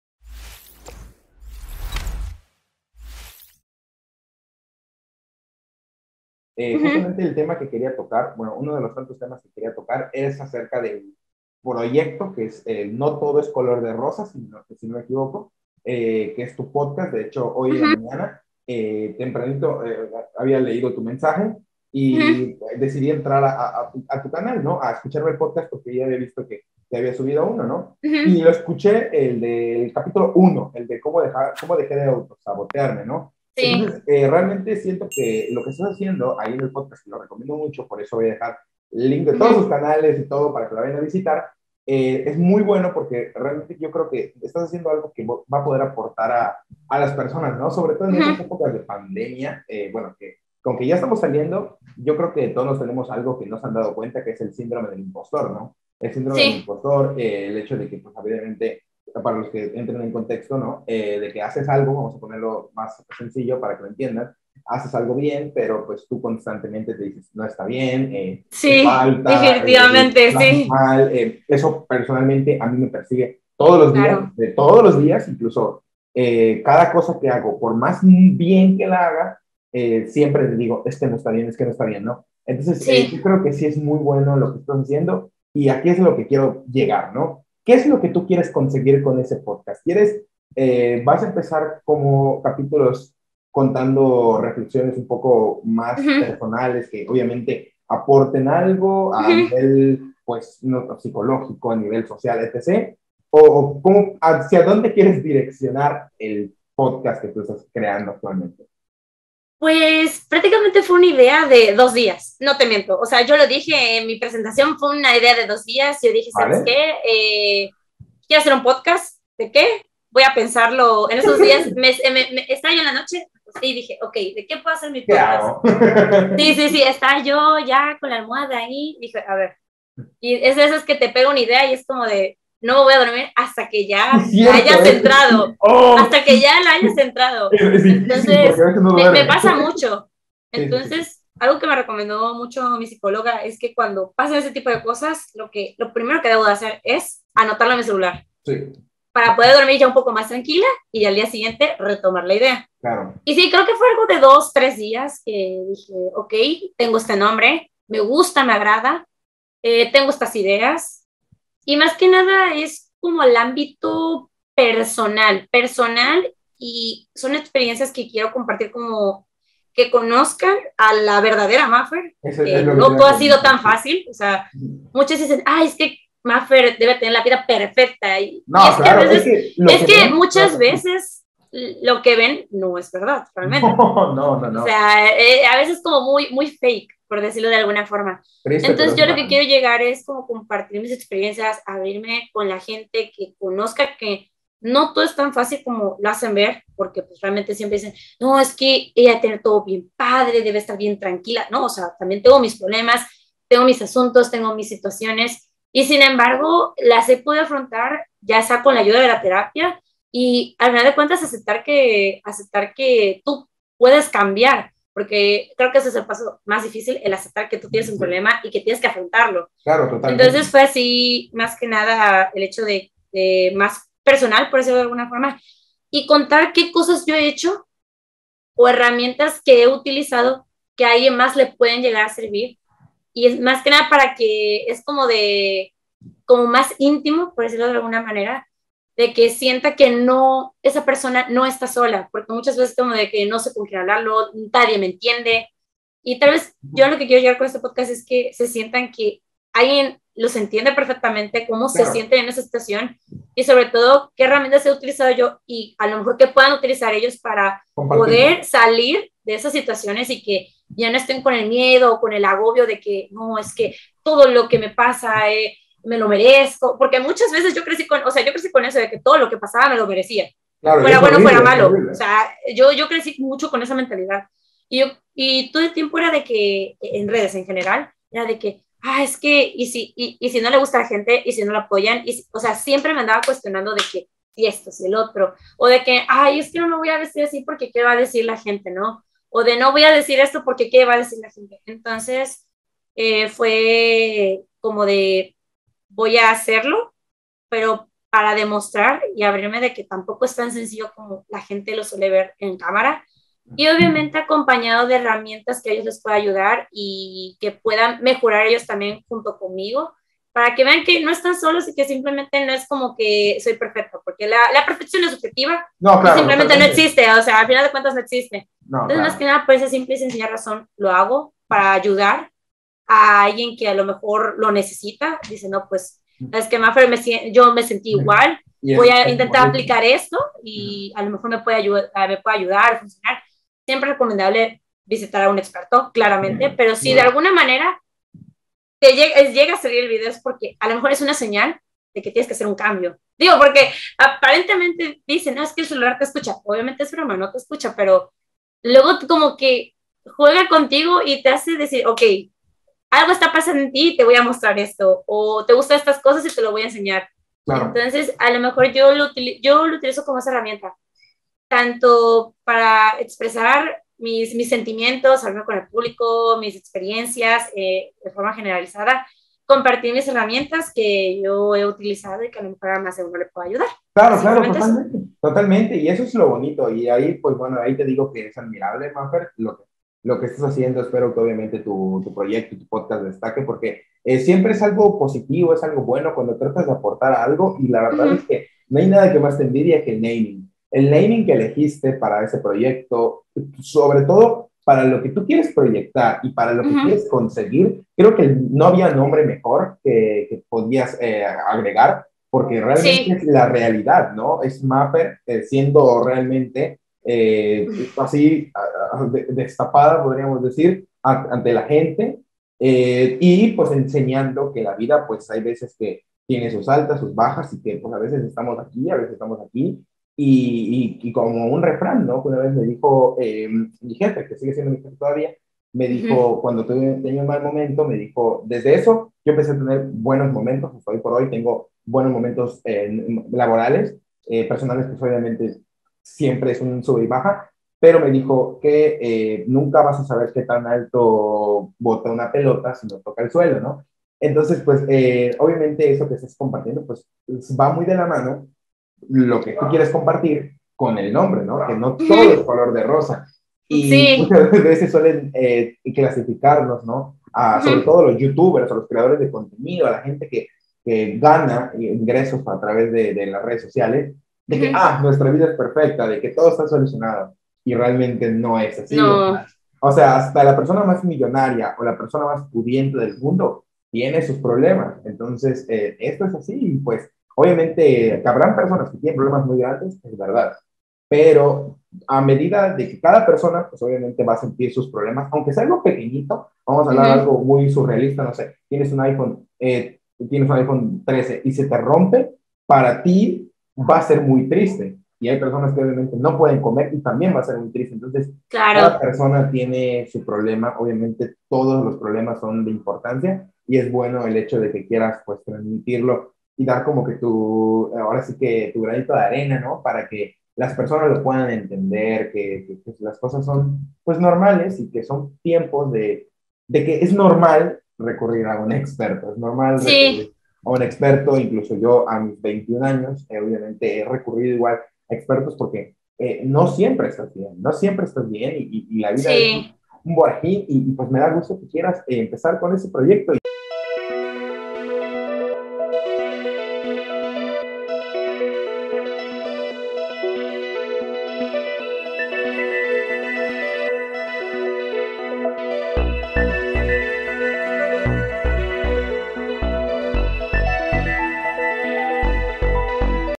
El nuevo proyecto de Mafer Tejeda 10: "No todo es color de rosa Podcast"
Mafer Tejeda 10 nos cuenta su nuevo proyecto en YouTube en el cual busca aportar cosas positivas para su audiencia.

Escucha el PodcAstroll en plataformas de audio: 🎧